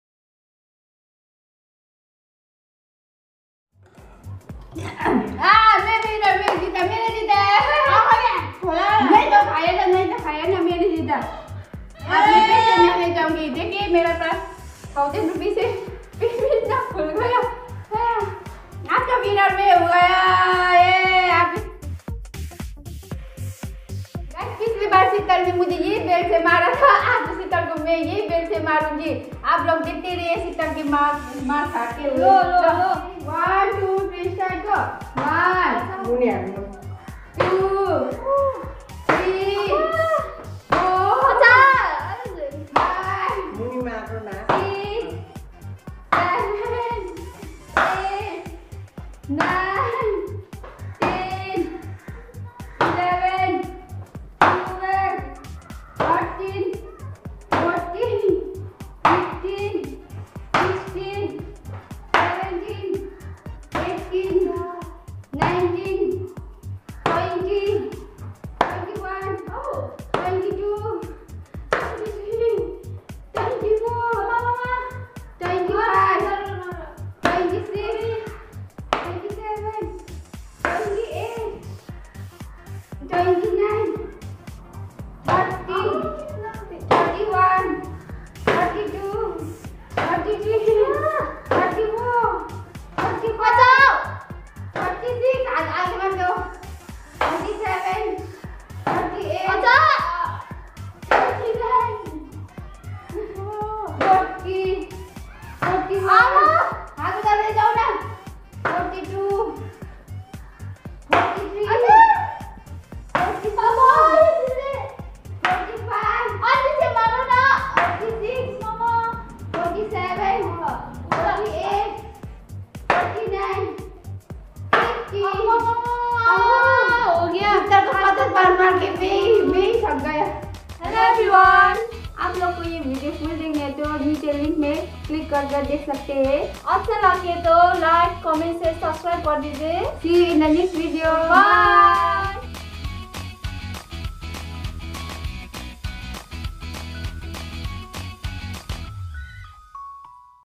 50 Sp@nkings Challenge 😂/ Food vs Spanking / Funny Game Challenge 🤪/ PriyaSheetalVlog 2.0
Hello Everyone welcome back to our channel PriyaSheetalVlog 2.0 we are priya ( 24yrs old ) & Sheetal ( 21 yrs old ), Today in this video we will be playing a game in which whoever finishes the food first without using hand will be a winner.

everything is scripted & no one is hurt during making our video

Funny Game
Funny Video
Food Challenge
Spank Challenge
Funny Game Challenge

e-mail Idawisrai@gmail.